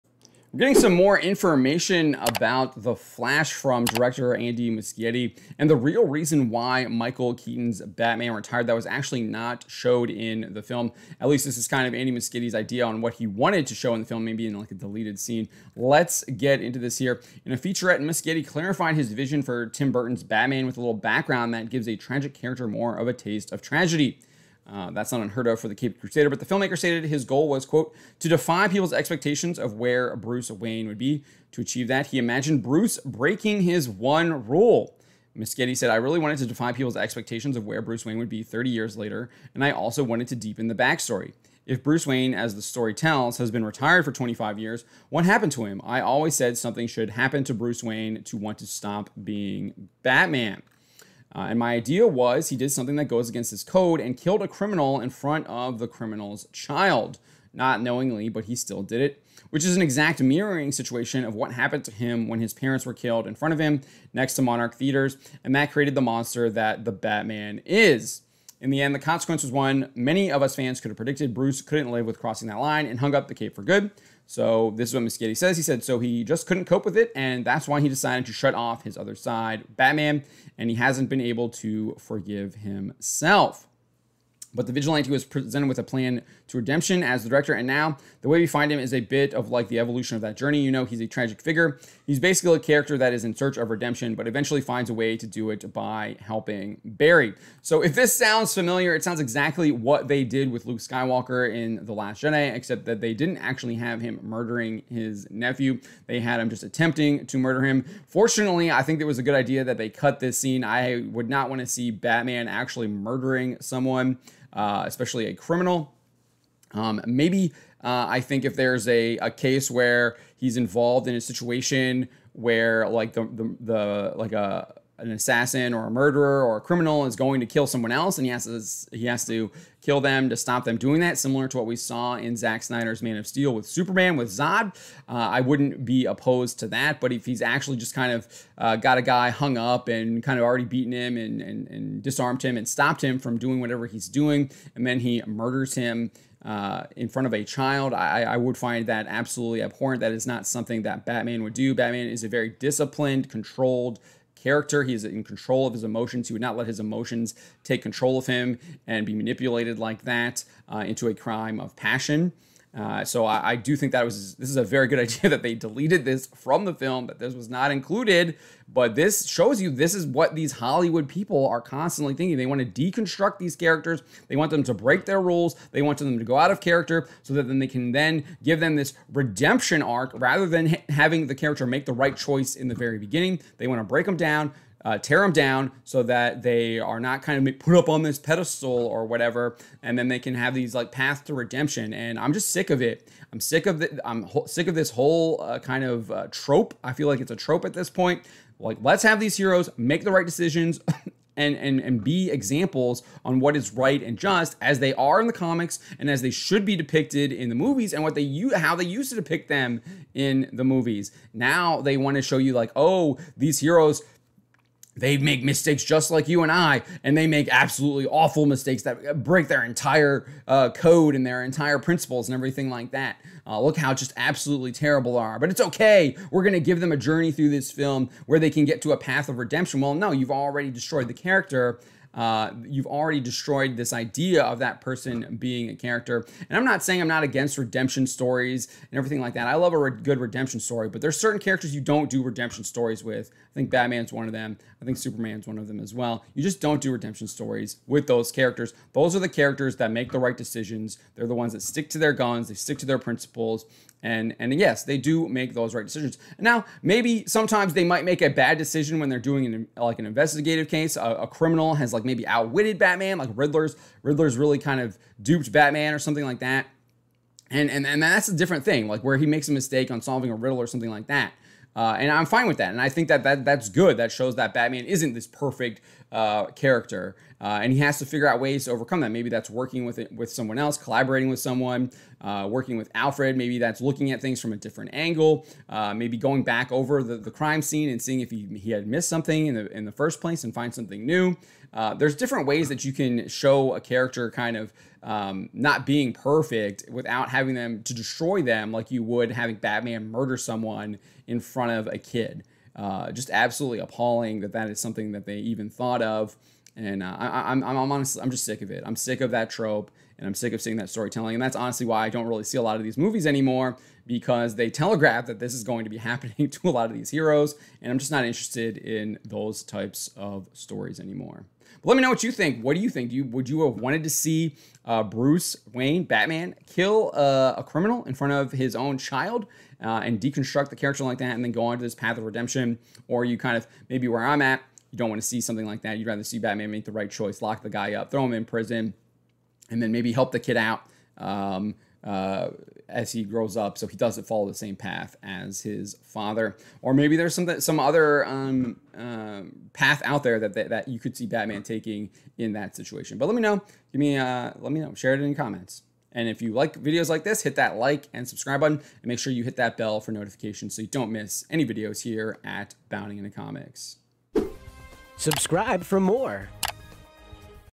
'The Flash' Director Reveals How He Wanted To DESTROY Michael Keaton's Batman
It would seem that Hollywood cannot resist destroying our heroes in order to replace them with their own VERY confused creations...